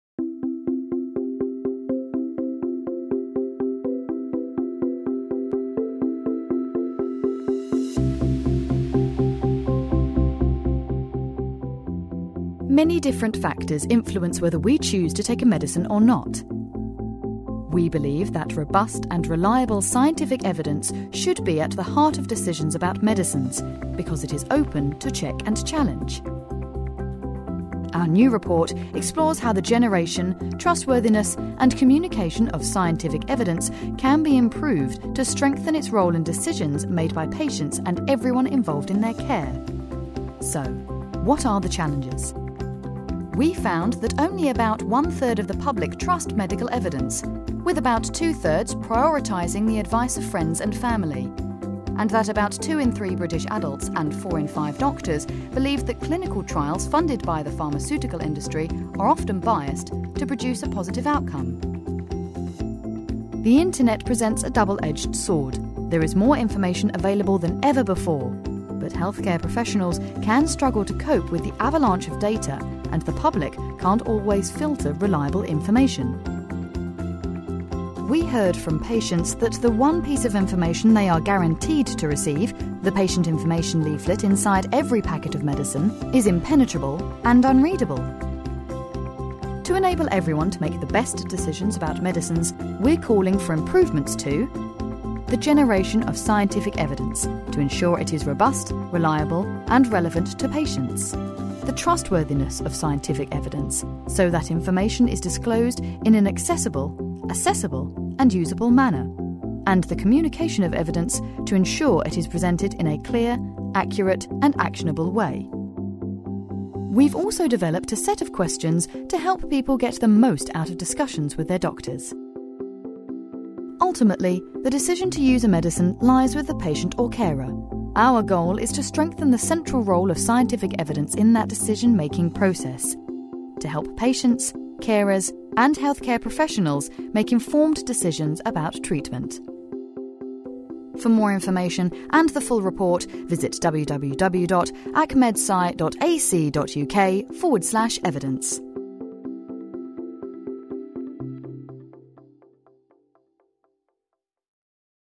Enhancing the use of scientific evidence to judge benefits and harms of medicines
Many different factors influence whether we choose to take a medicine or not. Our report explores how scientific evidence can be better generated and communicated, to enable everyone to make the best decisions about medicines

Here, we summarise the challenges that people face when seeking reliable medical evidence, and recommends how to strengthen the role of scientific evidence in the decision-making process, to help patients, carers and healthcare professionals make informed decisions about treatment.

This video is part of the 2017 Academy of Medical Sciences project, 'Enhancing the use of scientific evidence to judge the potential benefits and harms of medicines'. To find out more about the project,

We are the independent body in the UK representing the diversity of medical science. Our mission is to advance biomedical and health research and its translation into benefits for society.